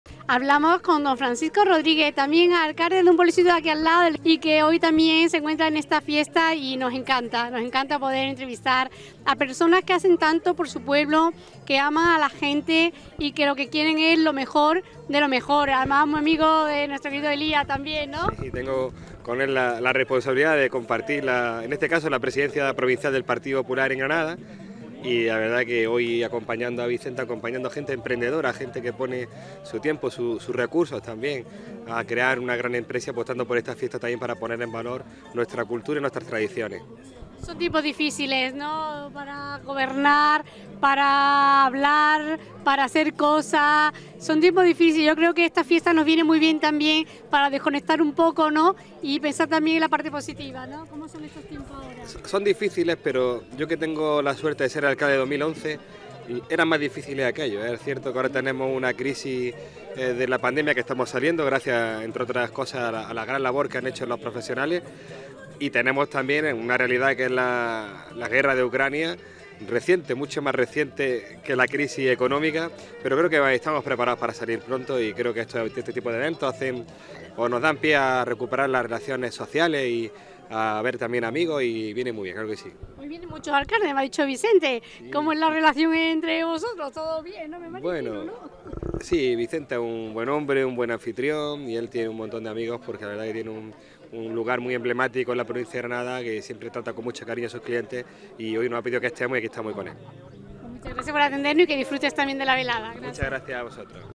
Entrevista de Helena Olaya a Francisco Rodríguez Guerrero, Alcalde de Alhendín Granada en Restauran
Entrevista de Helena Olaya a  Francisco Rodríguez Guerrero, Alcalde de Alhendín-Granada en Restaurante la Cumbre